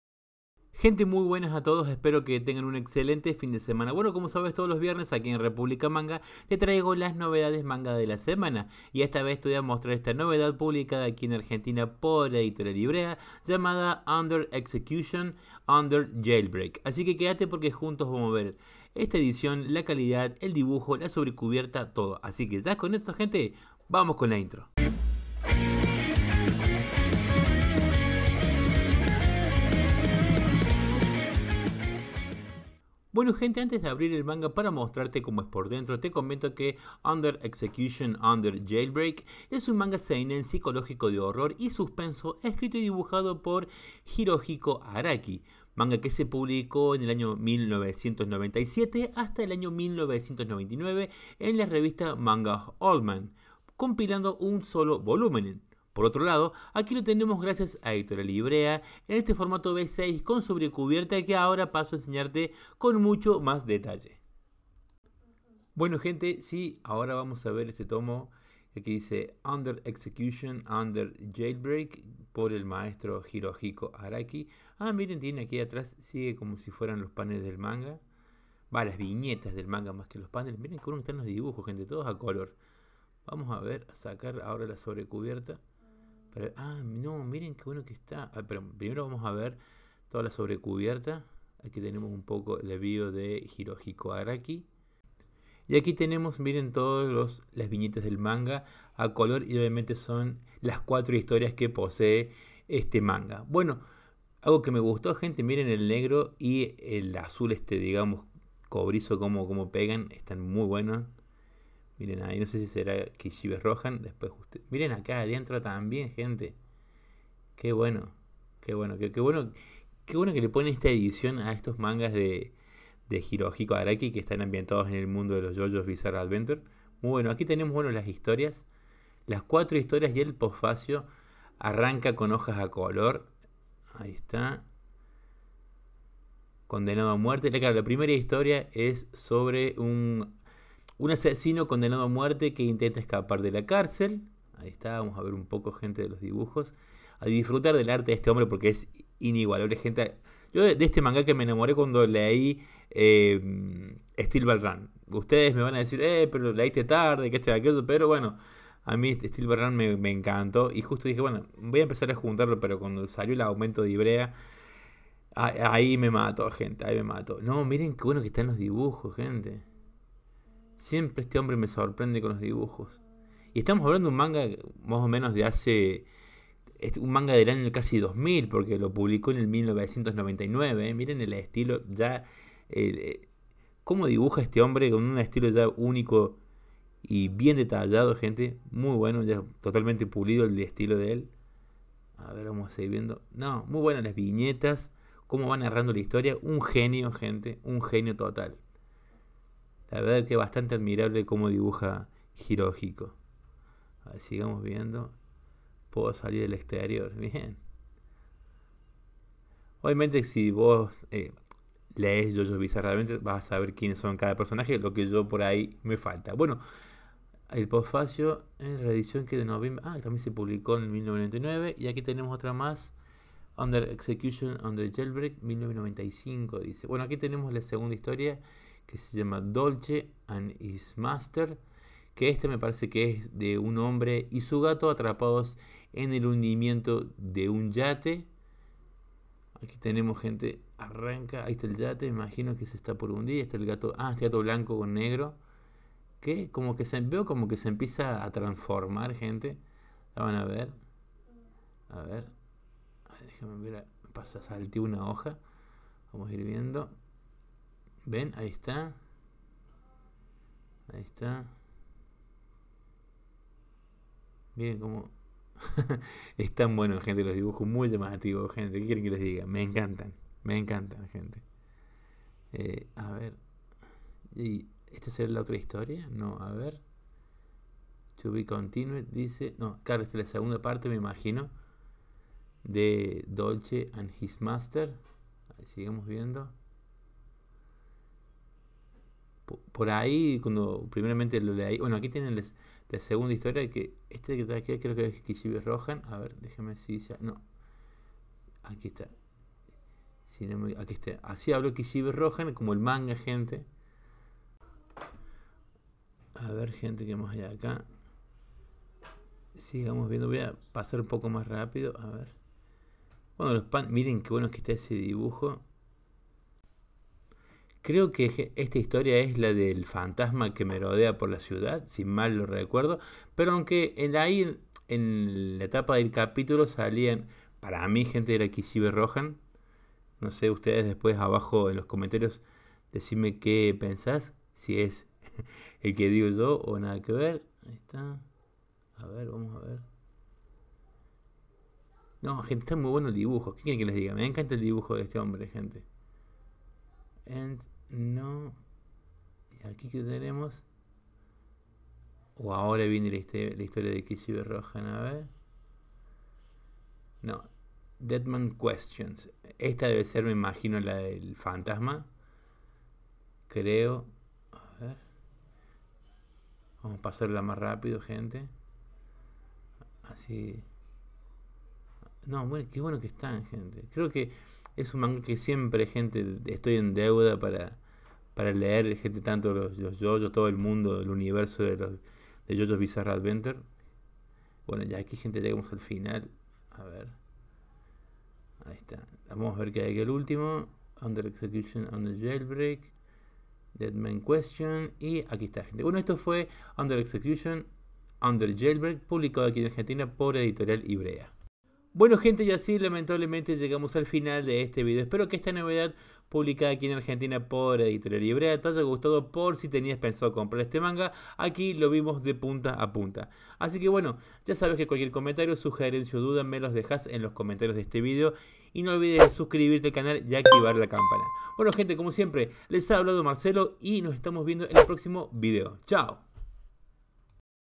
UNDER EXECUTION, UNDER JAILBREAK - IVREA
Gente muy buenas a todos y como todos los viernes te traemos las novedades manga de la semana en esta ocacion te muestro Under execution , under jailbreak publicado aqui en Argentina por editorial Ivrea, espero te guste y nos vemos en el proximo video!!